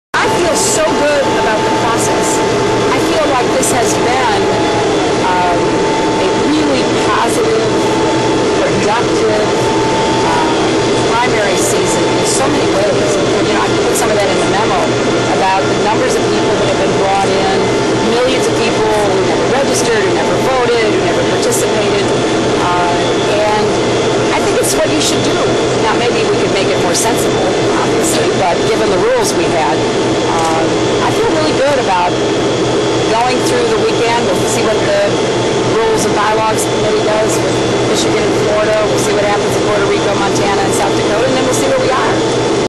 Clinton: Primary Season Has Been Productive
SelectPlusClinton: Primary Season Has Been ProductiveClinton: Primary Season Has Been ProductiveThe Associated PressSenator Hillary Clinton says she is proud the way the primary season has gone so far. She says changes could be made in the future, but it waiting to see how the process plays out before announcing what she will do next. (May 30)[Notes:ANCHOR VOICE] ___ ___, The Associated Press.( END ) ANCHOR VOICE: VIDEO PRODUCER: Kevin White VIDEO SOURCE: Pool VIDEO APPROVAL: VIDEO RESTRICTIONS: SCRIPT/WIRE SOURCE: